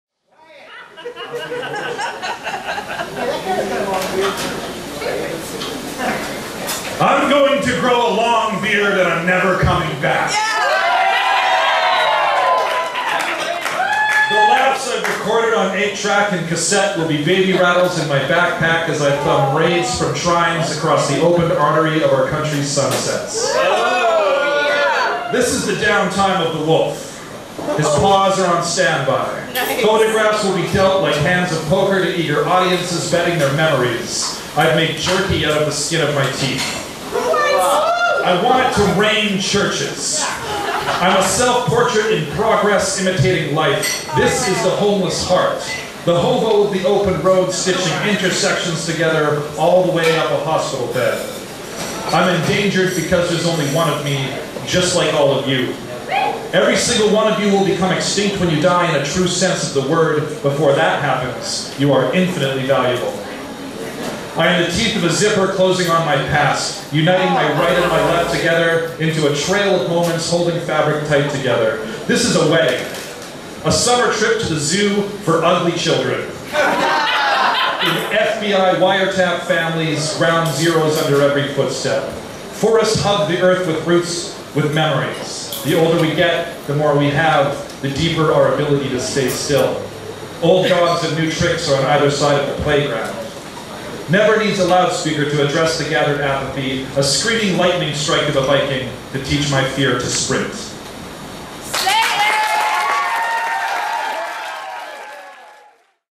Duncan Shields - Long Beard
Duncan Shields came in 4th place on the 16th of August with a score of 52.9 with his poem Long Beard.